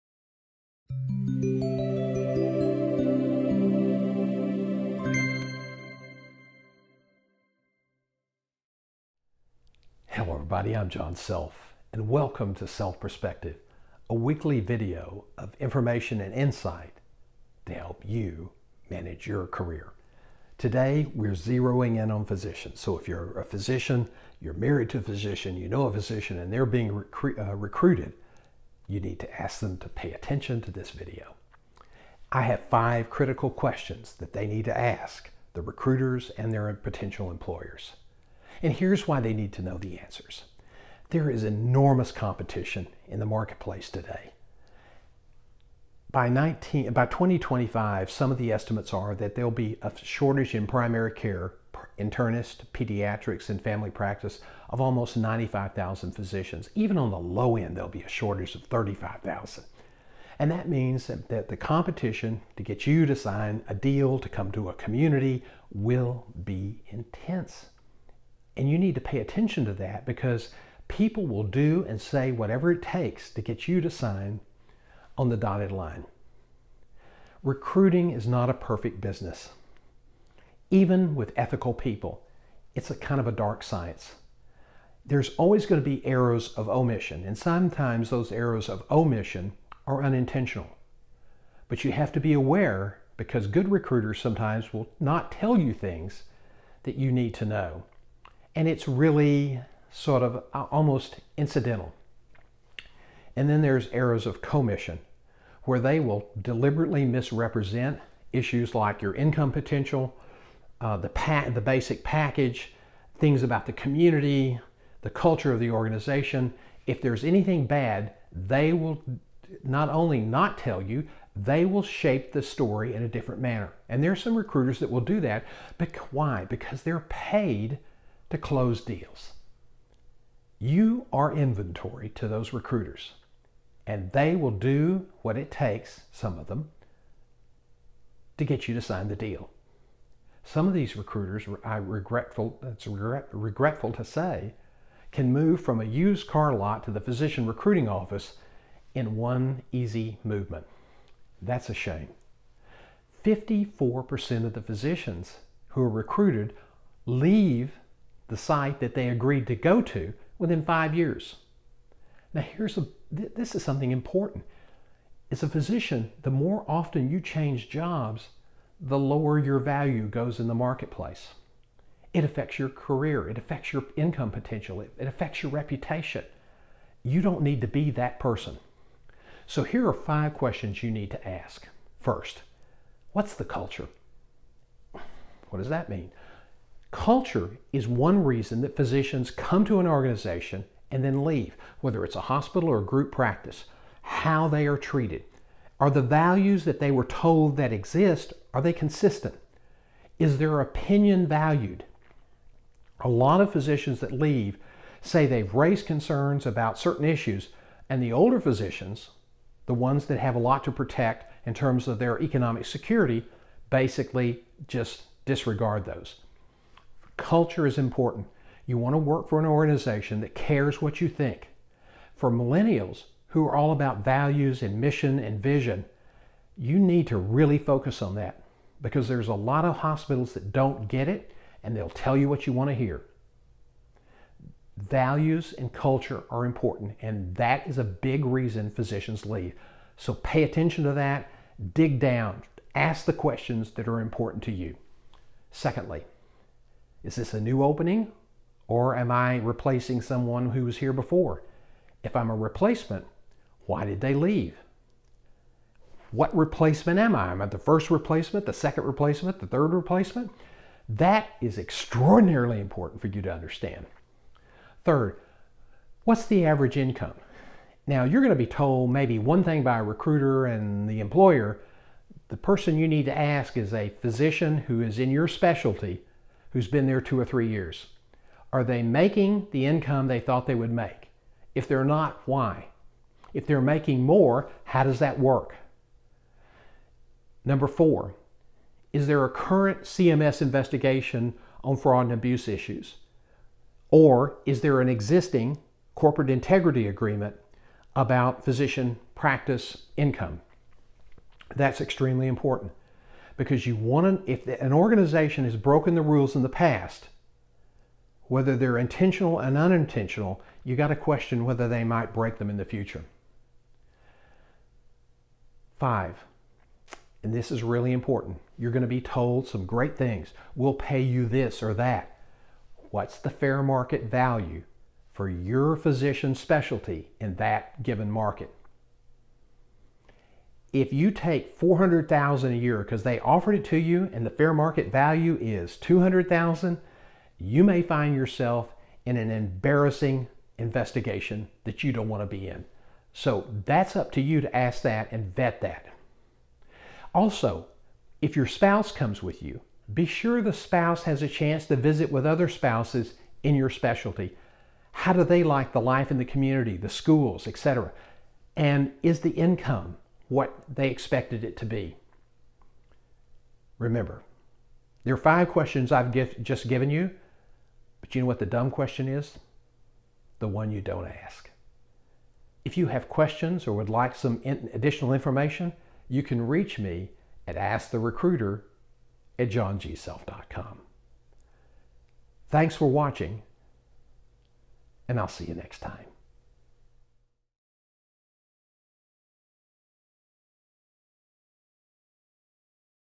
5 Questions Physicians Should Ask A Recruiter
By 2025 there will be a shortage of at least 35,000 physicians – some estimates say closer to 95,000. The competition will be intense so people will do and say whatever it takes to get you to sign on the dotted line. Here are five critical questions for physicians to ask recruiters and potential employers.